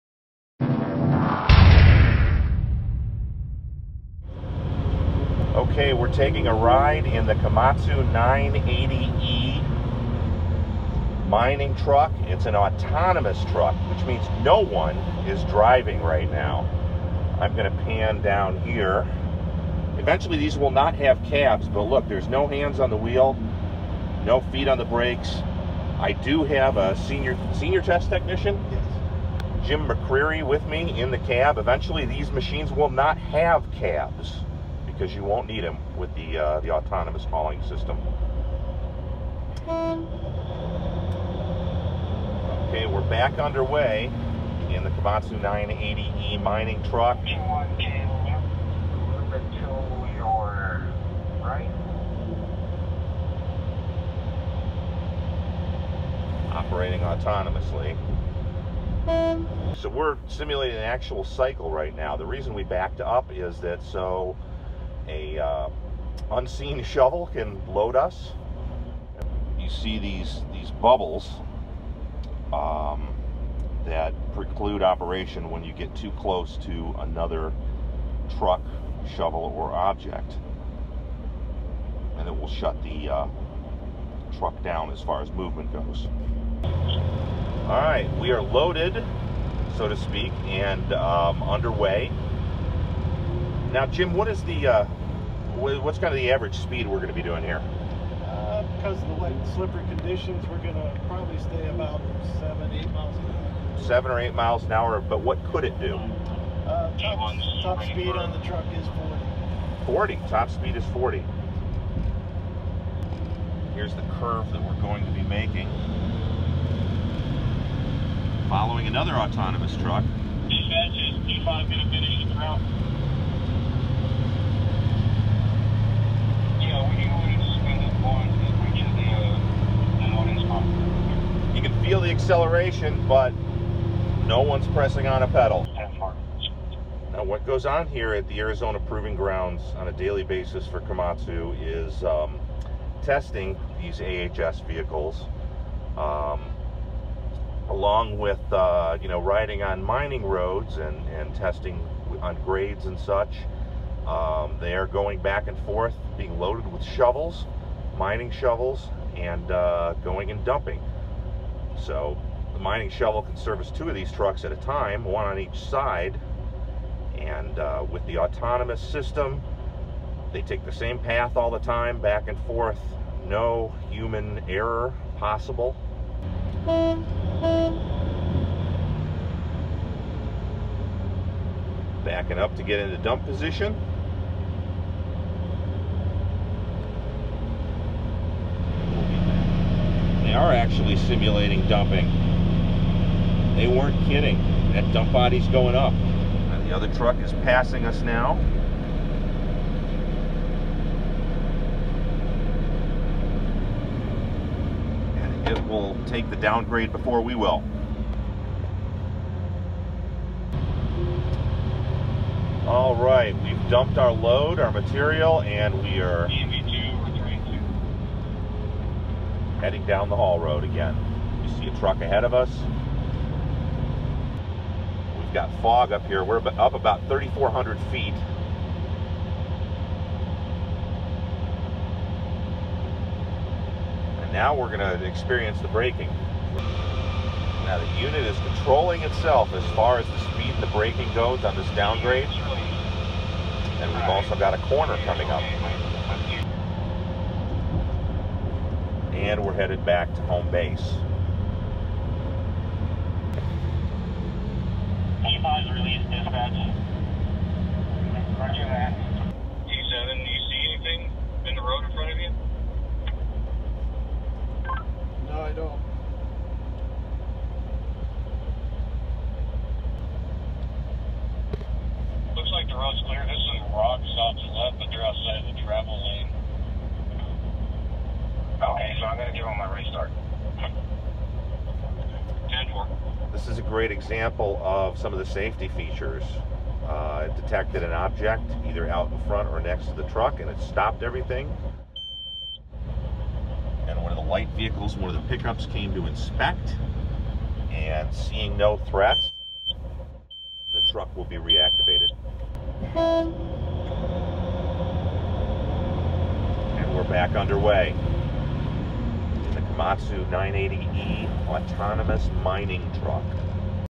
Ride Along in the Komatsu 980E Autonomous Mining Truck
Construction Equipment editors take a ride in the Komatsu 980E fully autonomous mining truck, offering a glimpse into the machine's daily work and functions straight from the operating seat.